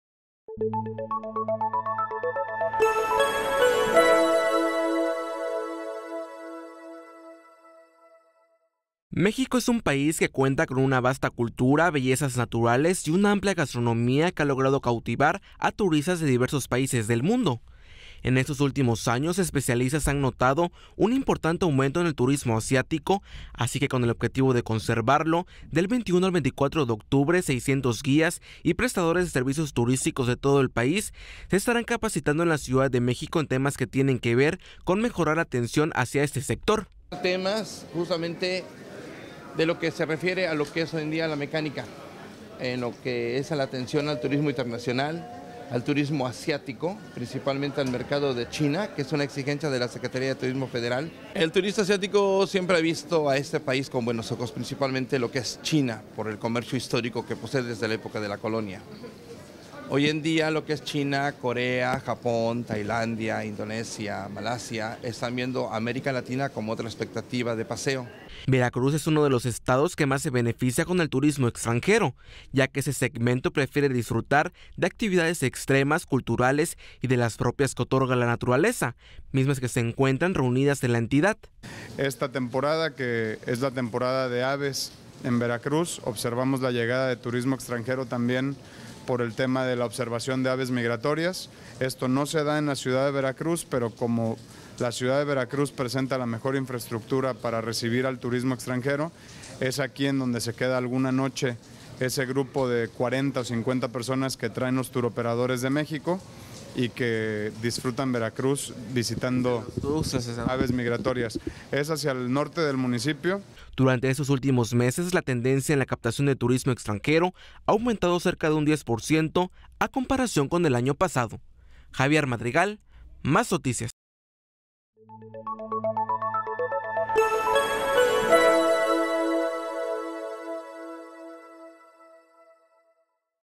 Guías turísticos se capacitan para atender a turistas extranjeros
México es un país  que cuenta con una vasta cultura, bellezas naturales  y una amplia gastronomía que ha logrado cautivar a turistas de diversos países del mundo.

En estos últimos años, especialistas han notado un importante aumento en el turismo asiático, así que con el objetivo de conservarlo, del 21 al 24 de octubre 600 guías y prestadores de servicios turísticos de todo el país se capacitarán en  la ciudad de México en temas que tienen que ver con mejorar la atención hacia este sector.